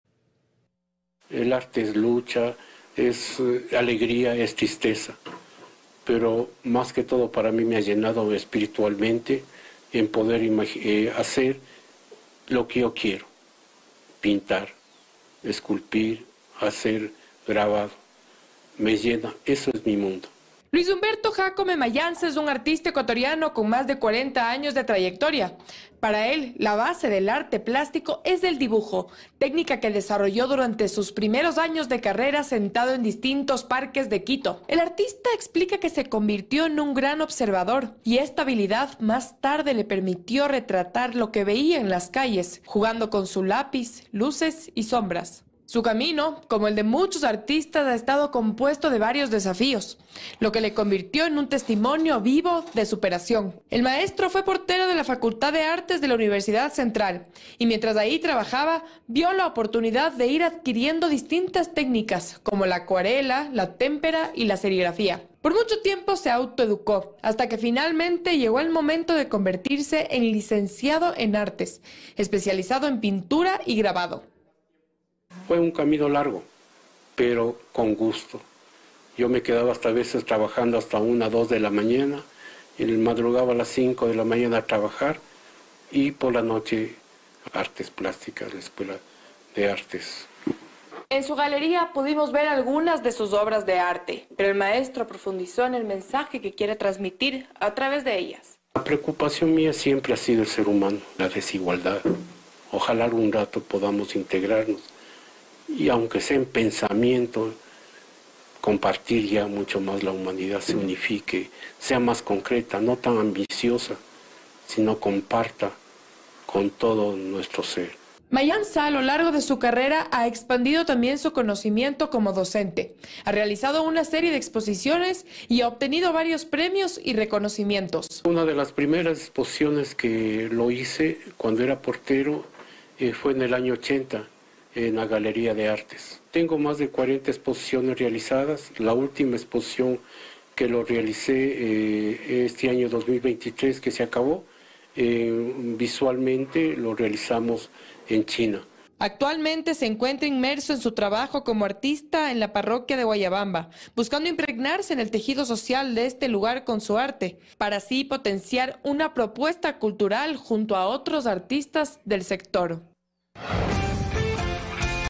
Luis Jácome es un artista ecuatoriano que muestra su versatilidad y originalidad en sus obras
Le traemos la historia de Luis Humberto Jácome Mayanza, artista ecuatoriano que se ha destacado por su talento, tanto a escala nacional, como internacional. Las distintas técnicas de Mayanza son muestra de la versatilidad y originalidad que envuelven a sus obras. Por: Bernarda Callejas